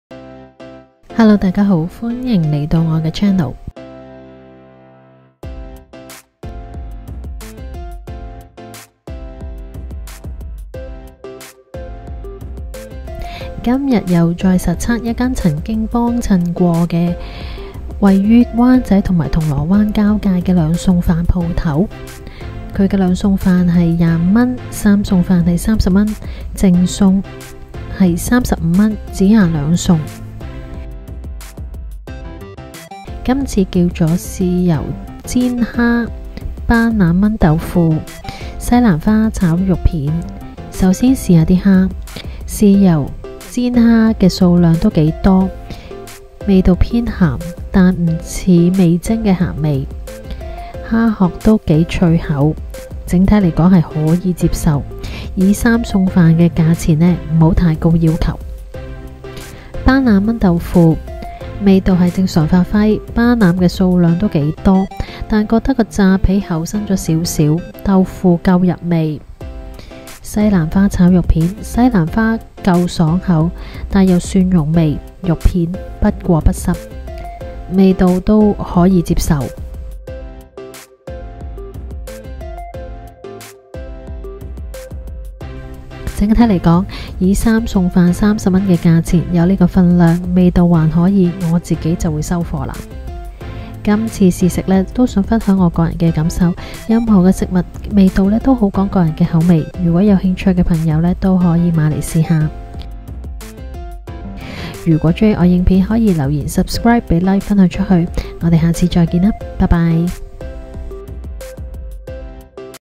【My Food Vlog🥑】再實測❗灣仔💲3⃣0⃣ 3餸飯❗自費・純分享
追蹤Follow｜IG: miss.rosy.sophie